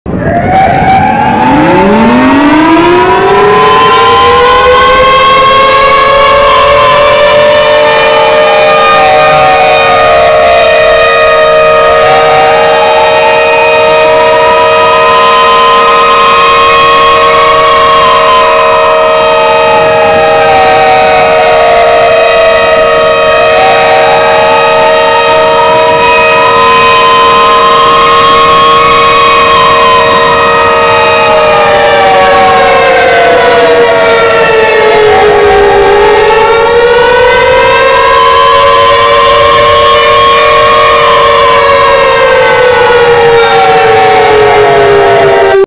The USA ball EAs alarm (2012)
This was played when the earthquake hitted capital American city